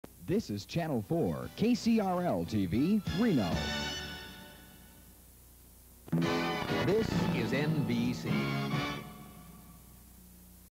KCRL channel 4 Reno, NV and NBC bumpers (1980)
Local station bumper and glitchy switchover to network feed. Call letters changed to KRNV in 1990.